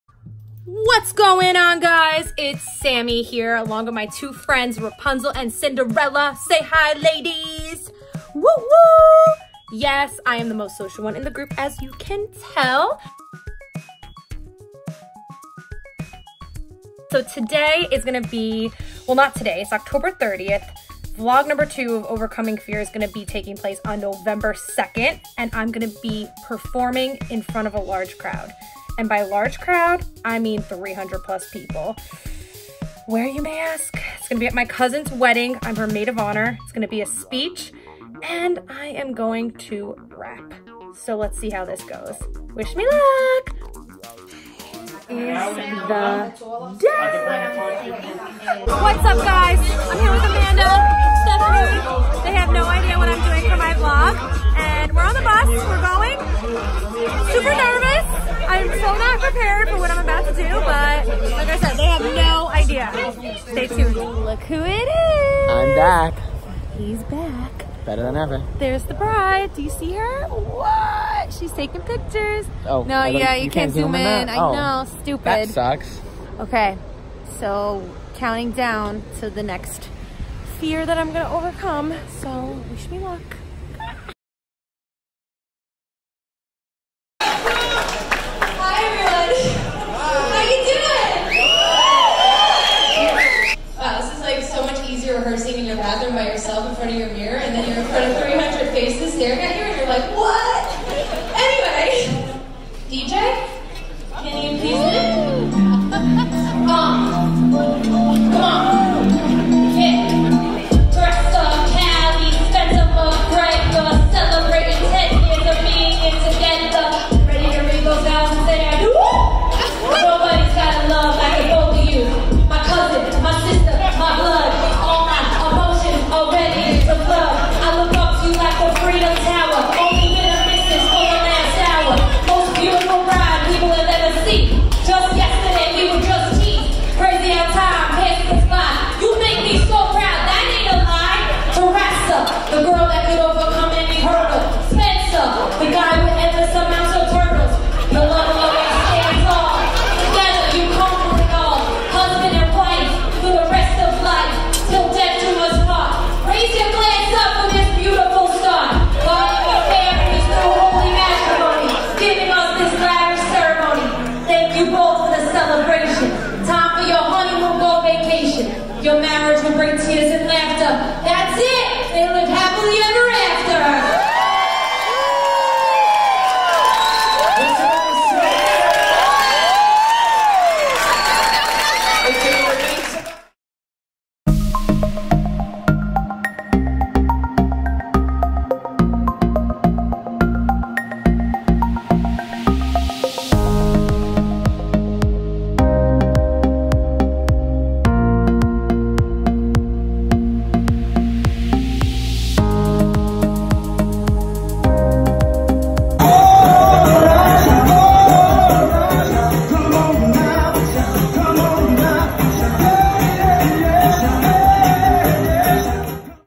Vlog #2 | See What I Did At My Cousin's Wedding!! | Nov. 2nd, 2019
Overcoming Fear in my second vlog at my cousin's wedding!!! It may have been scarier than skydiving to be honest:P Check it out!!